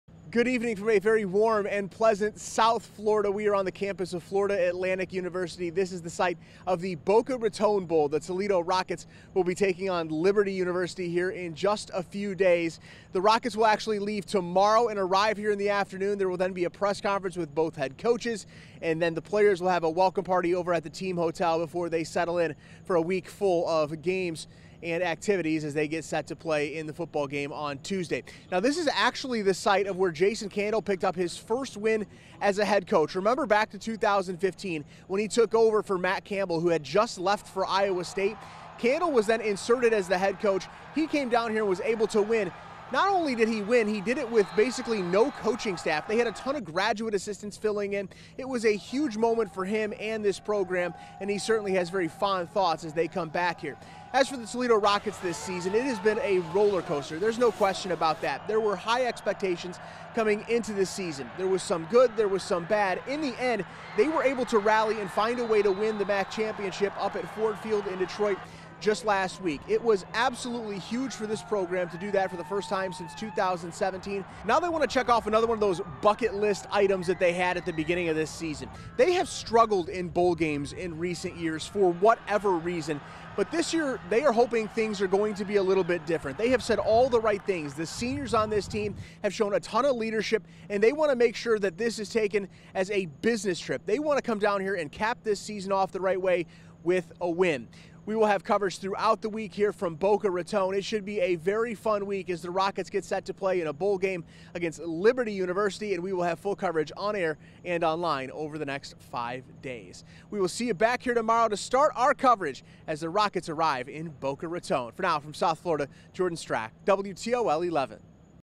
2022 Boca Raton Bowl: Toledo set to arrive Friday afternoon in Florida
The WTOL 11 sports team is bringing you coverage from Boca Raton as the Toledo Rockets are set to play against Liberty University in a bowl game on Tuesday, Dec. 20.